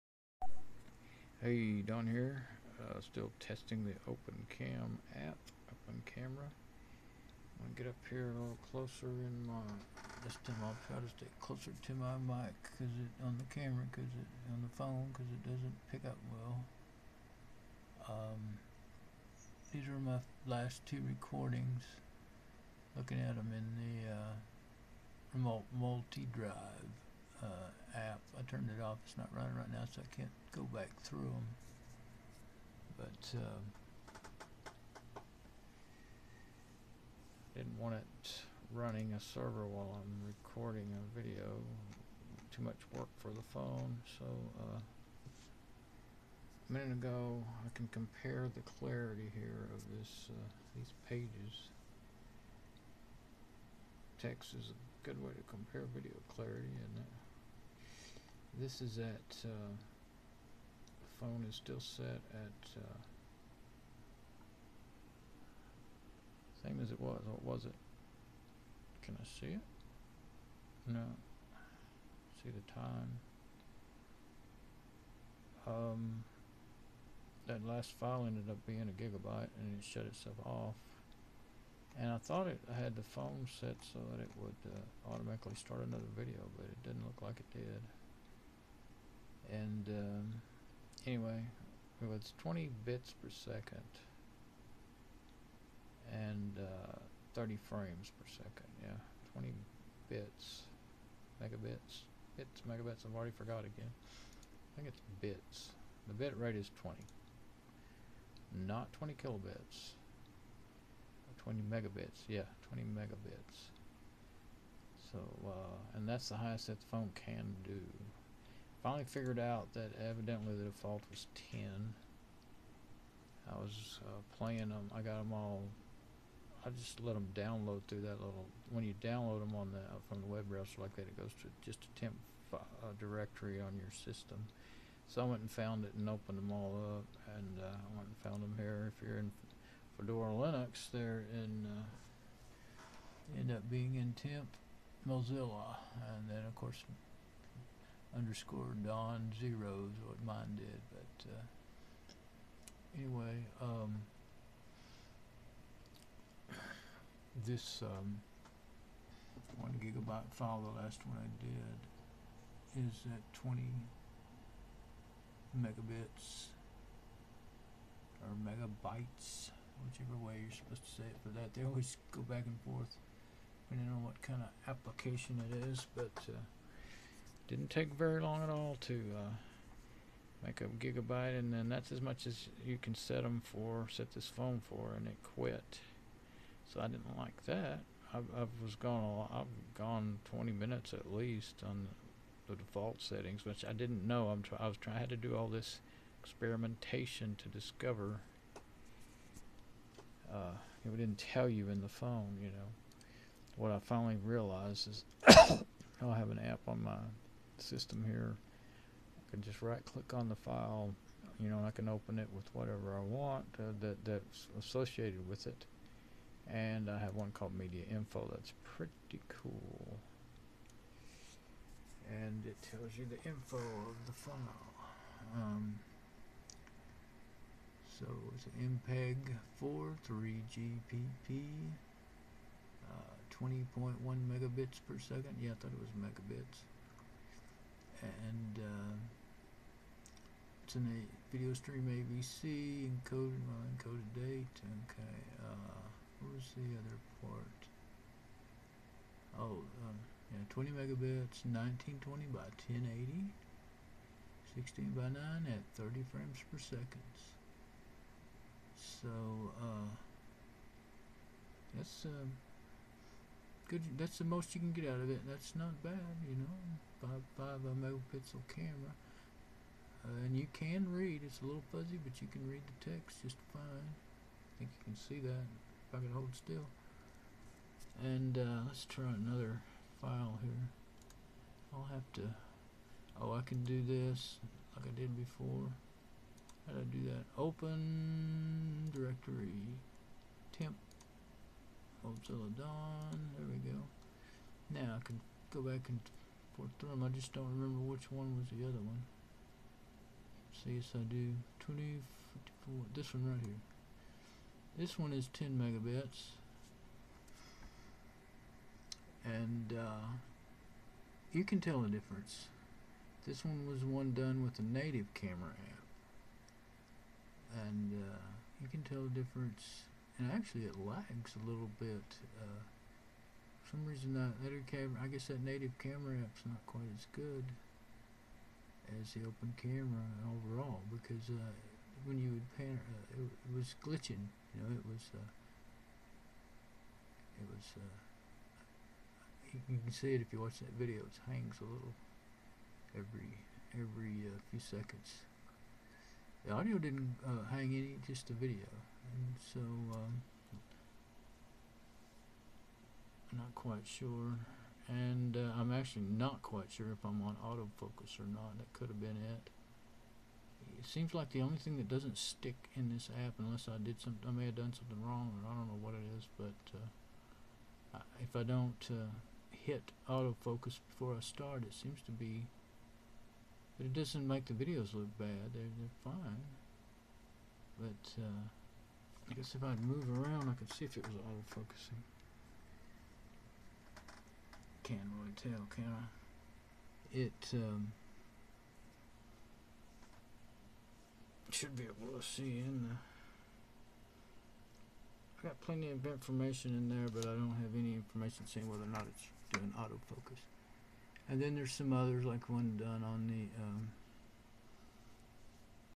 Don Bishop Testing Open Camera App Alcatel A845L Android Phone 5 Part Series Don Bishop
Testing Open Camera App, on my new Alcatel Pop Star A845L 4G LTE Android Phone. Video by Don Bishop These videos. were auto uploaded to Google Photos. From my Phone. Then I "uploaded" them from there to YouTube. So, I'm not sure, if the original quality, was kept or not. It's not clear, to me. Exactly what Resolution, Bit Rate etc, is. That Google, is allowing to be uploaded to Photos. These videos, look close to the originals, do. But, I can't tell for sure. withe the naked eye... Don YouTube Channel Description, for Dweb98: